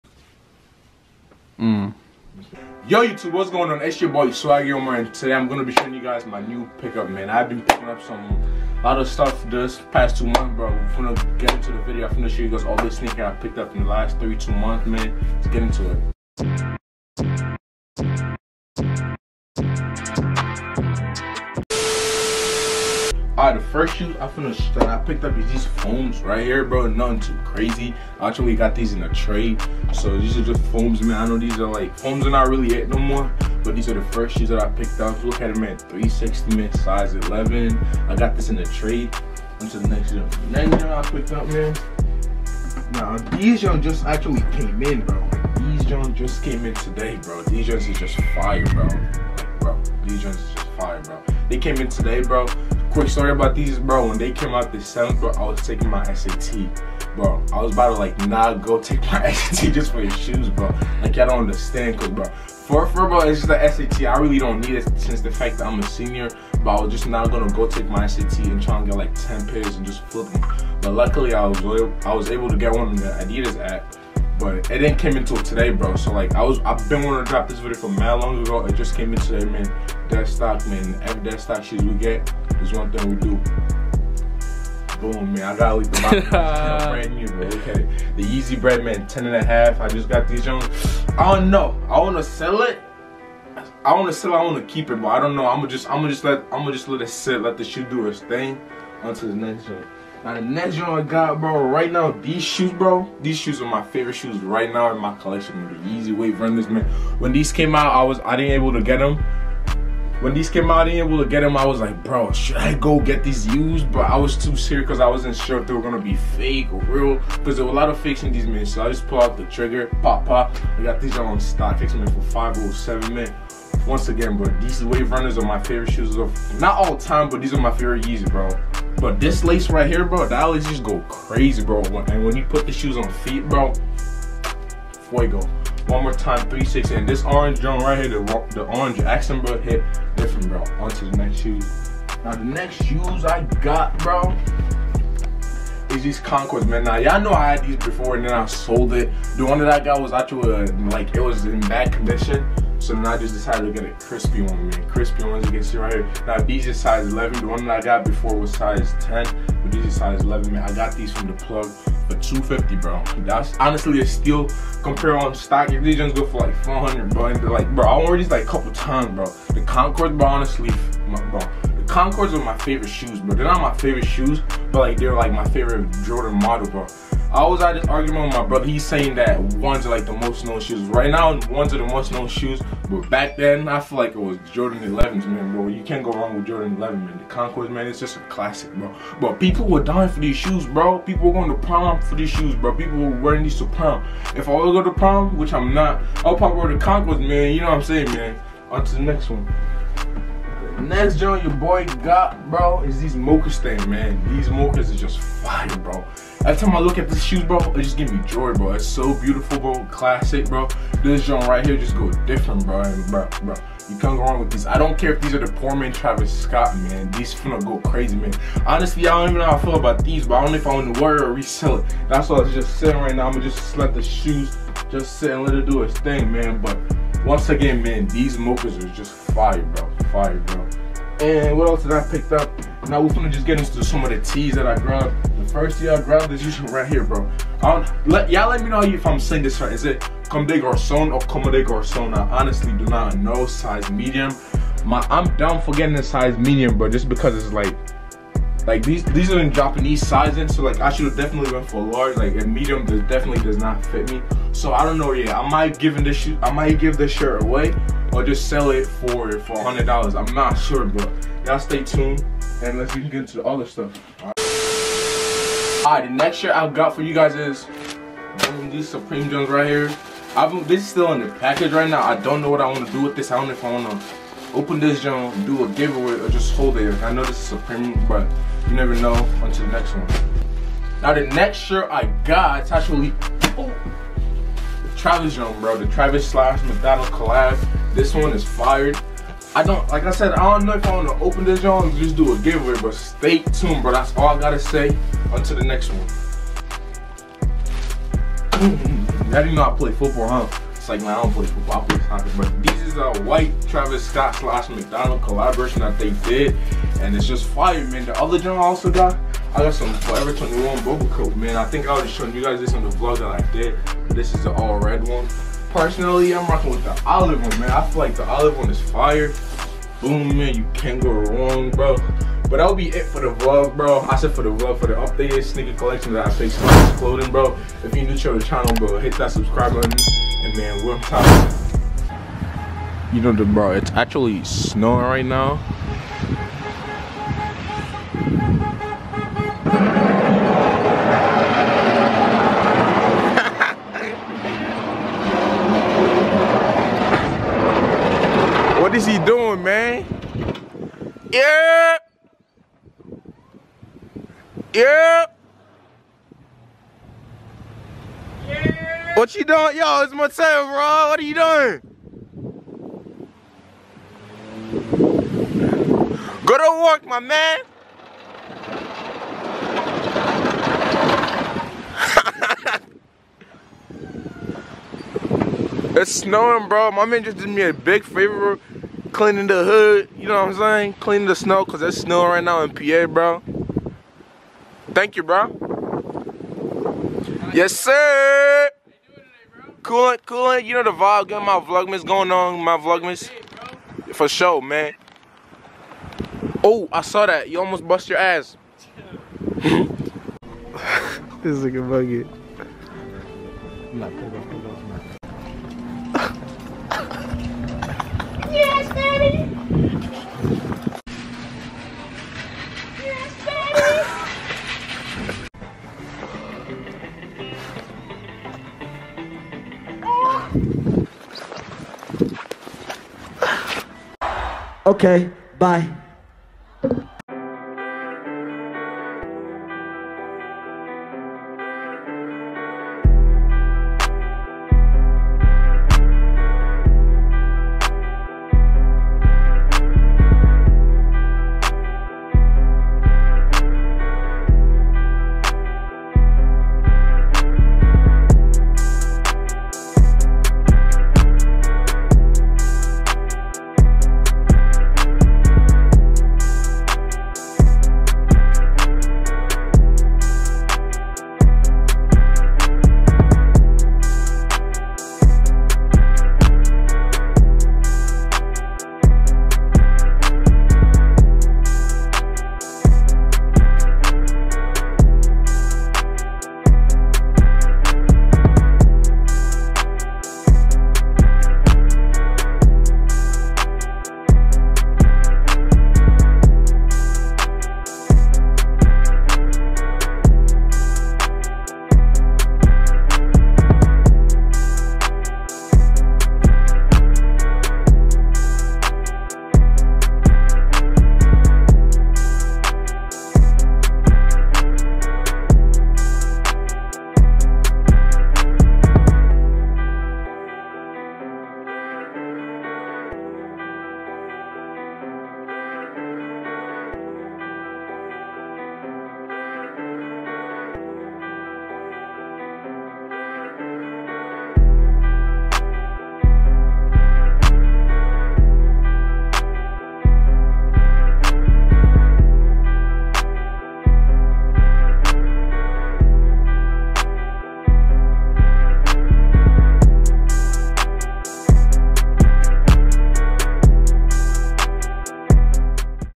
RECENT SNEAKER PICK-UPS 2020
Shoutout to everyone who watched the whole video! Share our channel and let’s hit 10k!🤘🏽🔥

In today's video I showed  you guys my recent 2020 pick-ups